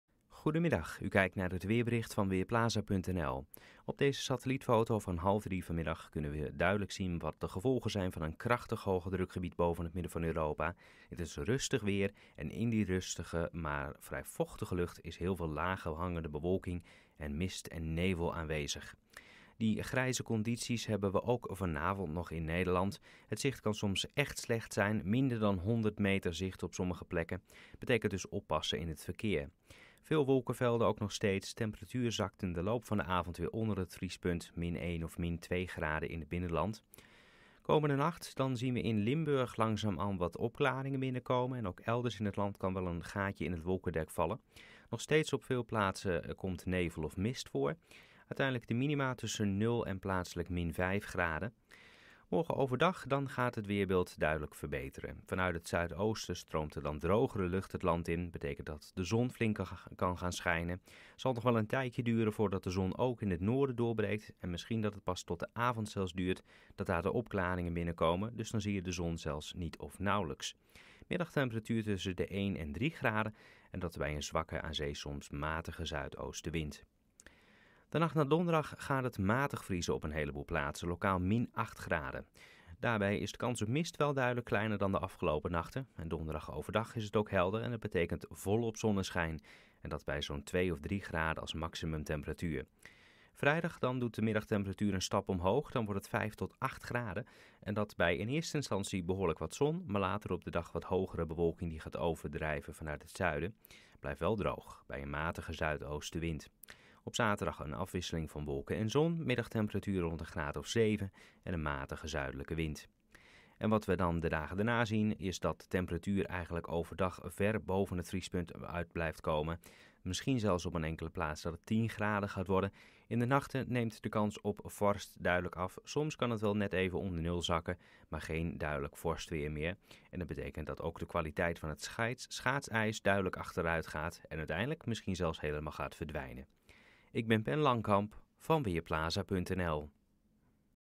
Weerbericht dinsdagmiddag: Vanaf woensdag zonniger
Het blijft bewolkt en nevelig of mistig op veel plaatsen. In het westen komen eerst de meeste opklaringen voor. Vannacht komen er vooral in het zuiden en zuidoosten langzaam meer opklaringen. De mist kan zich ook weer uitbreiden. Het minimum komt uit tussen 0 en -5 graden. Morgen overdag gaat de zon vanuit het zuidoosten geleidelijk doorbreken. In het westen en noorden klaart het pas tegen de avond op. Het wordt 1 tot 3 graden.